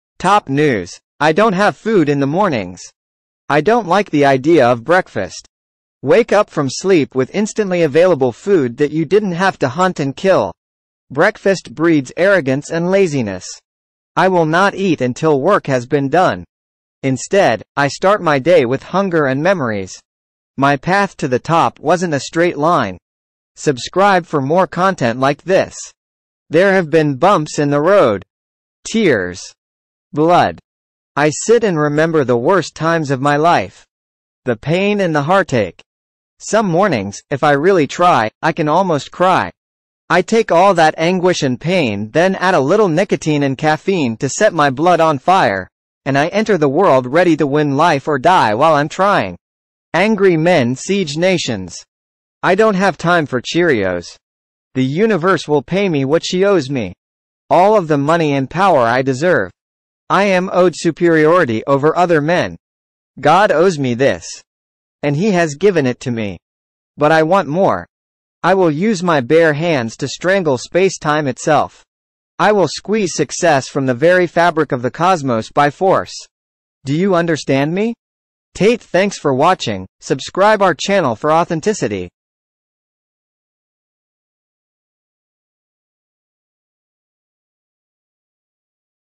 TOP NEWS I don’t have food in the mornings-Tate
I don’t have food in the mornings.

I don’t like the idea of breakfast.

Wake up from sleep with instantly available food that you didn’t have to hunt and kill.

Breakfast breeds arrogance and laziness.

I will not eat until work has been done.

Instead, I start my day with hunger and memories.

My path to the top wasn’t a straight line.

There have been bumps in the road.

Tears,

Blood.

I sit and remember the worst times of my life.

The pain and the heartache.

Some mornings, if I really try, I can almost cry.

I take all that anguish and pain then add a little nicotine and caffeine to set my blood on fire - and I enter the world ready to win life or die while I’m trying.

Angry men siege nations.

I don’t have time for cheerios.

The universe will pay me what she owes me.

All of the money and power I deserve.

I am owed superiority over other men. God owes me this. And he has given it to me.

But I want more.

I will use my bare hands to strangle space-time itself. I will squeeze success from the very fabric of the cosmos BY FORCE.

Do you understand me?